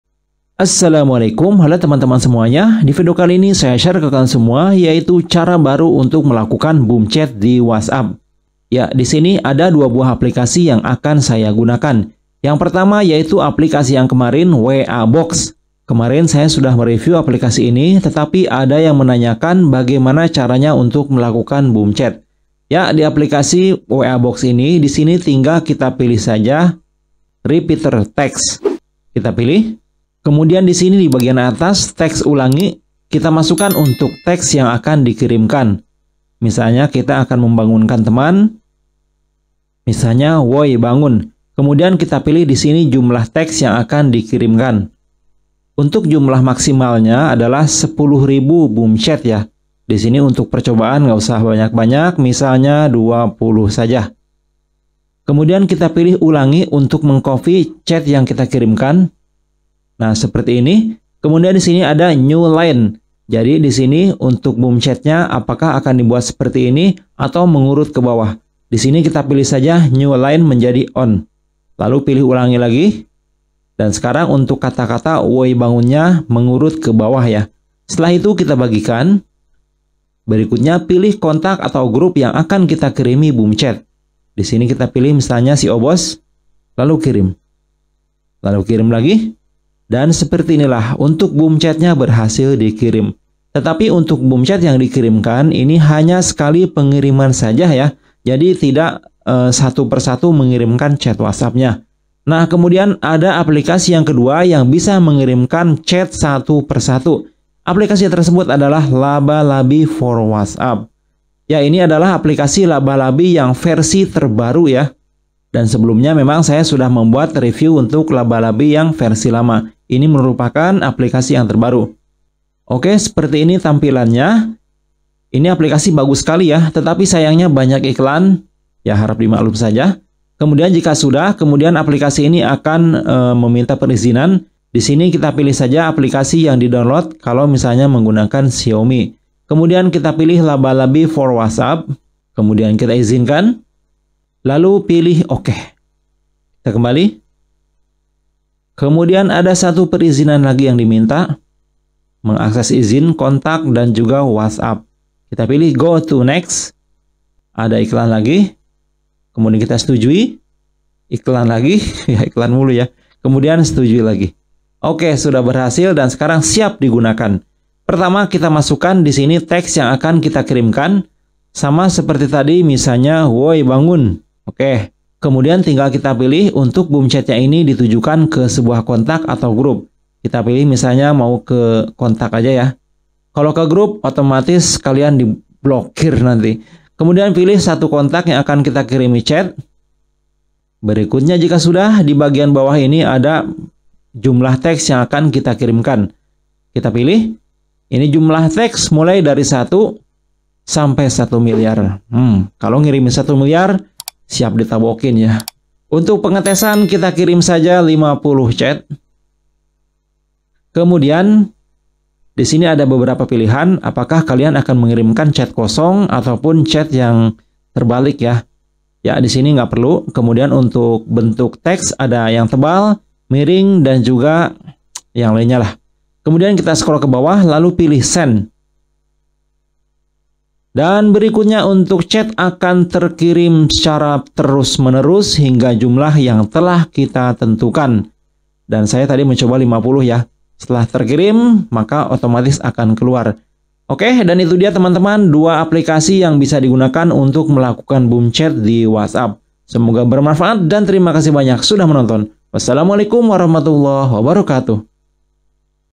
Cara Baru Membuat Boom Chat Whatsapp Hingga Sejuta Kali
Hai semuanya,di video ini saya share ke kalian semua yaitu cara baru untuk mengirimkan boom chat di whatsapp hingga satu juta kali.
Mengirimkan chat whatsapp dalam jumlah banyak atau disebut juga boom chat bisa dilakukan secara otomatis mulai dari 10 hingga satu juta kali pengiriman chat.

Namun perlu diperhatikan,ada beberapa hal yang harus dipertimbangkan ketika kita akan mengirimkan spam chat whatsapp dalam jumlah banyak,diantaranya jangan sampai kamu menyinggung perasaan teman atau mengganggu ketenangan dkarenakan pengiriman chat yang terus menerus.

Oke untuk membuat boom chat whatsapp ini,ada dua aplikasi yang bisa kalian gunakan,yang pertama adalah aplikasi wa box,dan yang kedua menggunakan aplikasi laba labi,untuk link download ada di akhir video saya ini,sjadi silahkan kalian simak videonya yah.

Terima Kasih